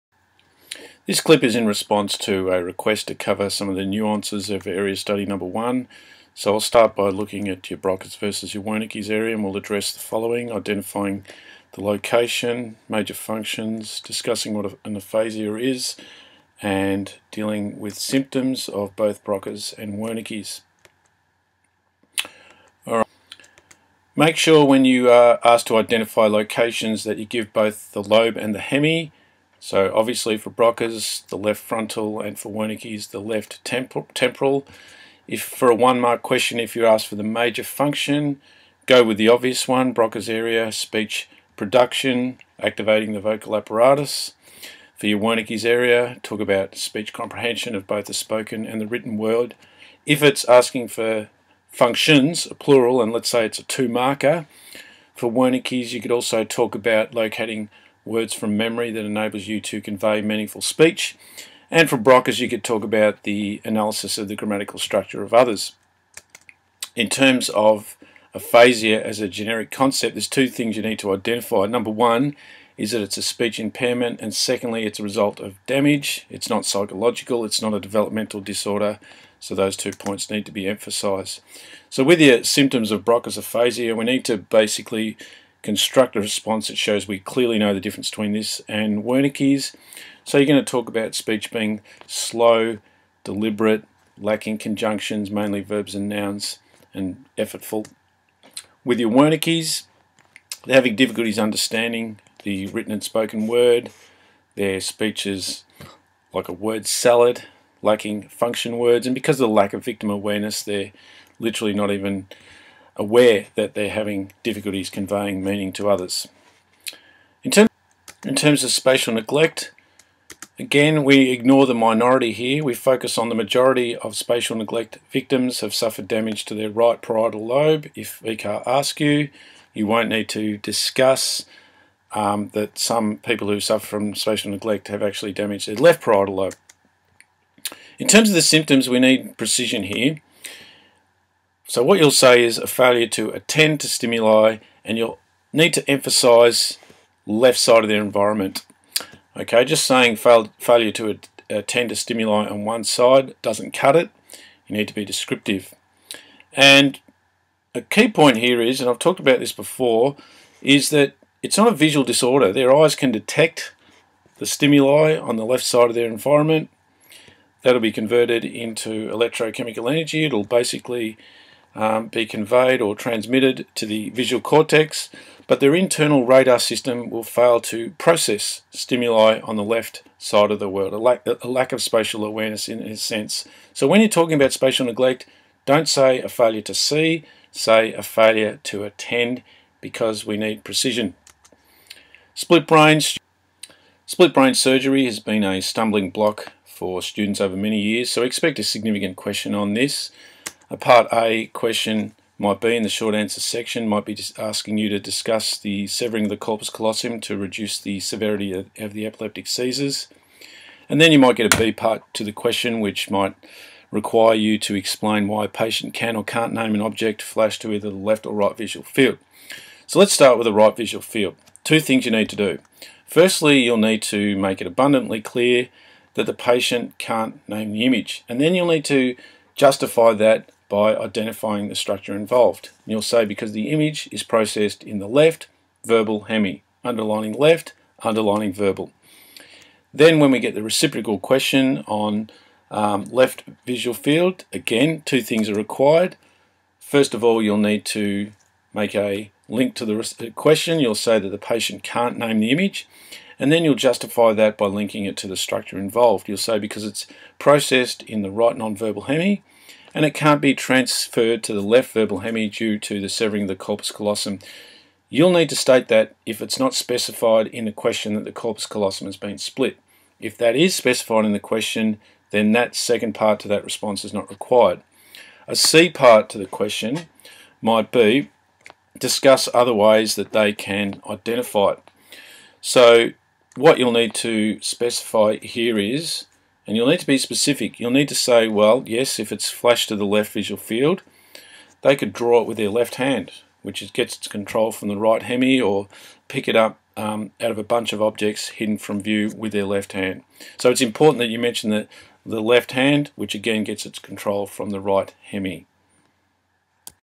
Brain and Body - VCE Psych exam pointers
Split brain, Spatial neglect, Broca's & Wernike's aphasia are covered in this revision clip.